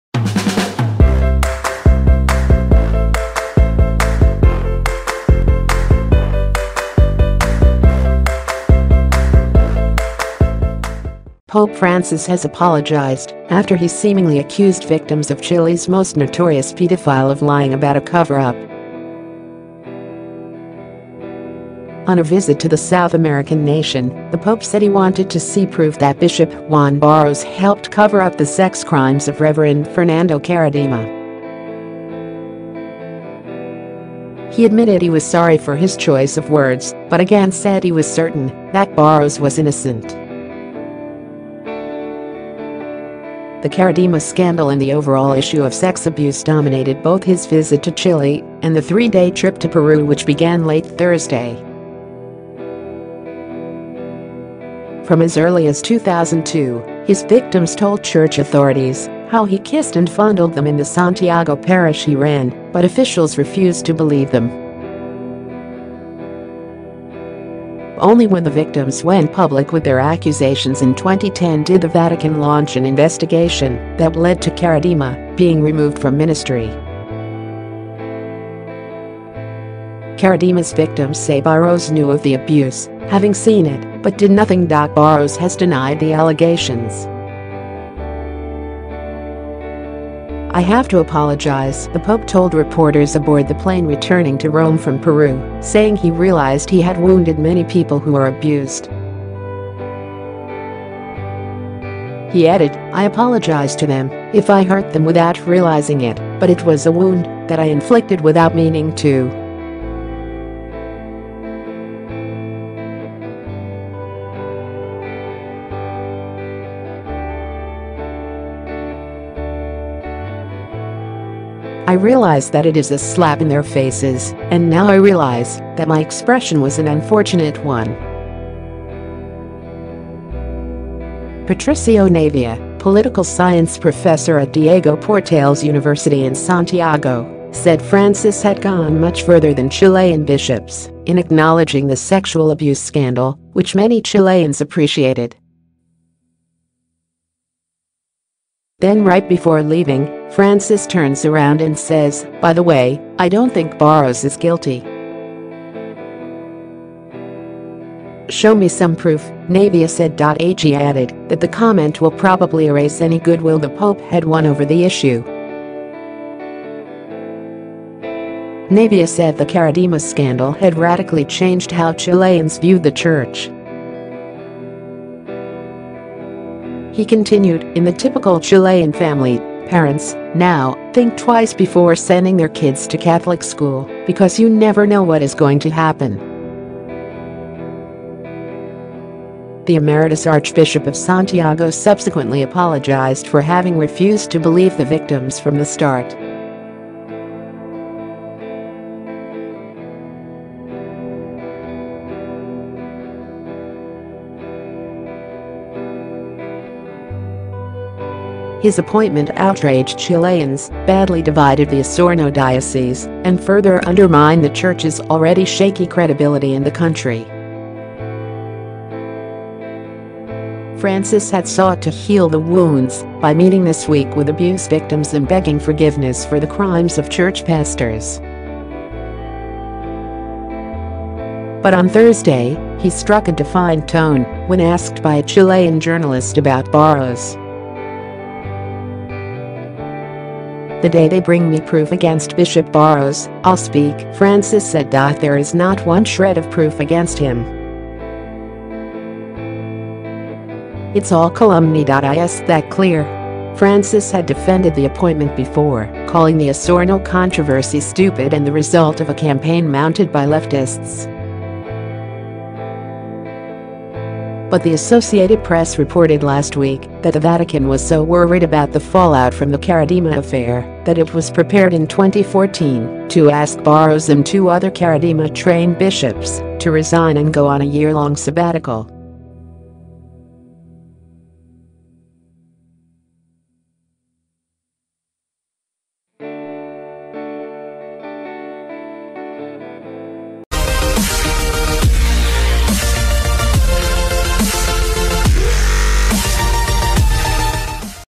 Pope Francis apologises for defending paedo priests in Chile - News 247
POPE Francis has apologised after he seemingly accused victims of Chile’s most notorious paedophile of lying about a cover-up.On a visit to the South American nation, the Pope said he wanted to see proof that Bishop Juan Barros helped cover up the sex crimes of Reverend Fernando Karadima.He admitted he was sorry for his choice of words but again said he was certain that Barros was innocent.The Karadima scandal and the overall issue of sex abuse dominated both his visit to Chile and the three-day trip to Peru which began late Thursday.From as early as 2002, his victims told church authorities how he kissed and fondled them in the Santiago parish he ran - but officials refused to believe them.Only when the victims went public with their accusations in 2010 did the Vatican launch an investigation that led to Karadima being removed from ministry.Karadima’s victims say Barros knew of the abuse, having seen it, but did nothing.Barros has denied the allegations."I have to apologise," the Pope told reporters aboard the plane returning to Rome from Peru, saying he realised he had "wounded many people who were abused." He added: "I apologise to them if I hurt them without realising it, but it was a wound that I inflicted without meaning to."I know how much they [abuse victims] suffer in hearing the pope say to them 'bring me a letter with the proof'."I realise that it is a slap in their faces, and now I realise that my expression was an unfortunate one." Patricio Navia, political science professor at Diego Portales University in Santiago, said Francis had gone much further than Chilean bishops in acknowledging the sexual abuse scandal, which many Chileans appreciated."Then right before leaving, Francis turns around and says: 'By the way, I don’t think Barros is guilty.Show me some proof,'" Navia said.He added that the comment will probably erase any good will the pope had won over the issue.Navia said the Karadima scandal had radically changed how Chileans view the church.He continued: "In the typical Chilean family, parents (now) think twice before sending their kids to Catholic school because you never know what is going to happen." The emeritus archbishop of Santiago subsequently apologised for having refused to believe the victims from the start.Francis reopened the wounds of the scandal in 2015 when he named Barros, a protege of Karadima, as bishop of the southern diocese of Osorno.His appointment outraged Chileans, badly divided the Osorno diocese and further undermined the church’s already shaky credibility in the country.Francis had sought to heal the wounds by meeting this week with abuse victims and begging forgiveness for the crimes of church pastors.But on Thursday, he struck a defiant tone when asked by a Chilean journalist about Barros."The day they bring me proof against Bishop Barros, I’ll speak," Francis said."There is not one shred of proof against him.It’s all calumny.Is that clear?”  Francis had defended the appointment before, calling